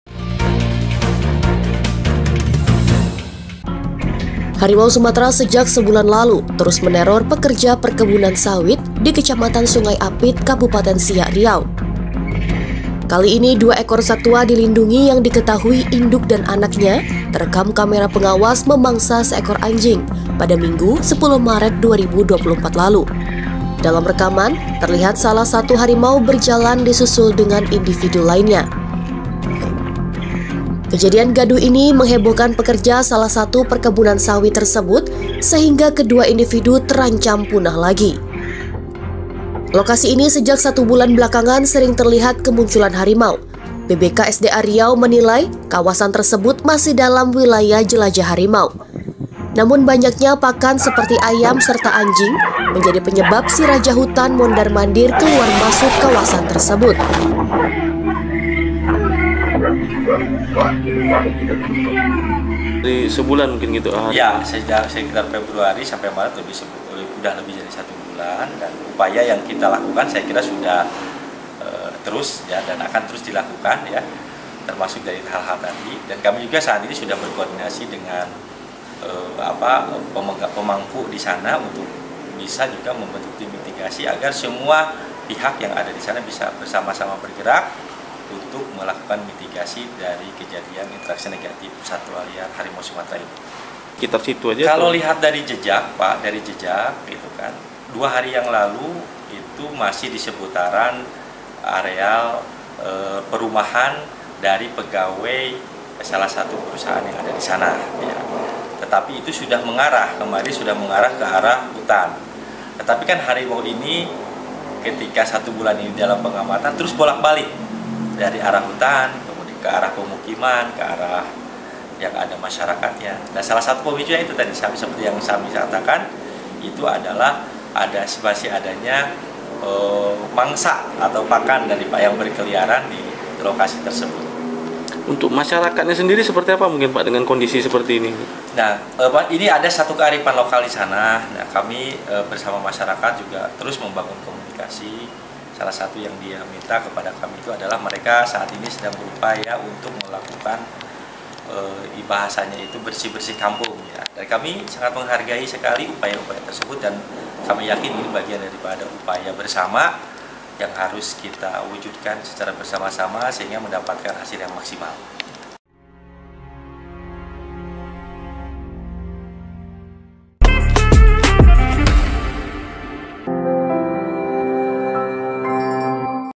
Terekam CCTV Harimau Sumatera Mangsa Anjing Warga
Harimau sumatera sejak sebulan lalu terus meneror pekerja perkebunan sawit di Kecamatan Sungai Apit, Kabupaten Siak, Riau. Kali ini, dua ekor satwa dilindungi yang diketahui induk dan anaknya terekam kamera pengawas memangsa seekor anjing, Minggu ( 10/03/24 ) lalu.

Dalam rekaman, terlihat salah satu harimau berjalan disusul dengan individu lainnya.

Ke dua harimau sumatera ini merupakan individu yang sama yang sempat terekam kamera perangkap beberapa waktu lalu.

Kemunculan satwa panthera tigris sumatrae ini membuat gaduh anjing dan sontak memancing si belang.

Kejadian gaduh ini menghebohkan pekerja salah satu perkebunan sawit tersebut sehingga ke dua individu terancam punah pergi.

Lokasi ini sejak satu bulan belakangan sering terlihat kemunculan harimau. BBKSDA Riau menilai kawasan tersebut masih dalam wilayah jelajah harimau, namun banyaknya pakan seperti ayam serta anjing menjadi penyebab si raja hutan mondar mandir keluar masuk kawasan tersebut.

Dari pengamatan dilokasi, BBKSDA Riau melihat jejak harimau mengarah ke kawasan hutan yang berbatasan langsung dengan barak pekerja.

BBKSDA Riau meminta warga untuk mengkandangkan hewan ternak agar tidak memancing harimau.